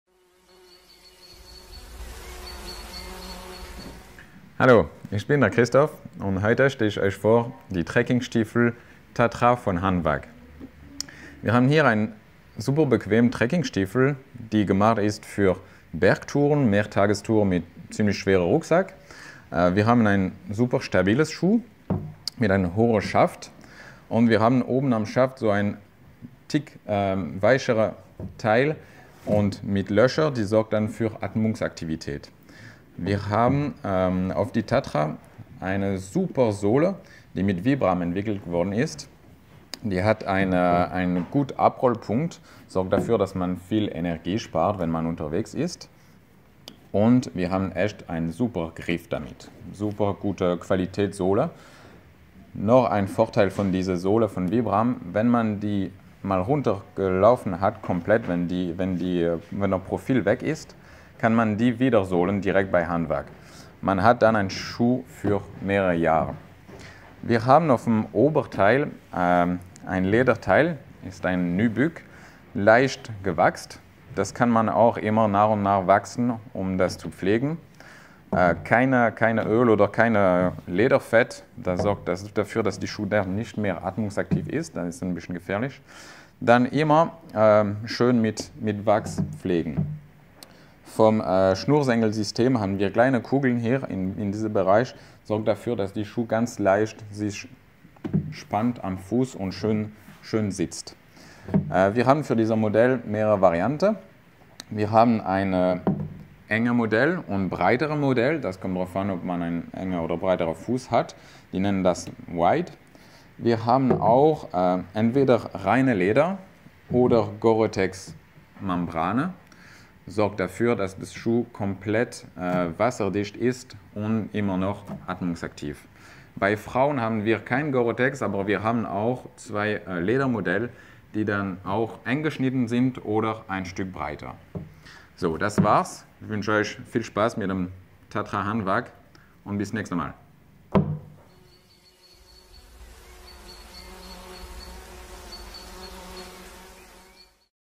Wanderschuhe Test - Produkttest Trekkingstiefel Wanderschuh Hanwag Tatra
Trekkingstiefel Tatra von Hanwag vorgestellt von Christophe. Eigenschaften und Vorteile dieses außergewöhnlichen Trekkingschuhs präsentiert in diesem Produktvideo.

Doorout.com - Outdoor erleben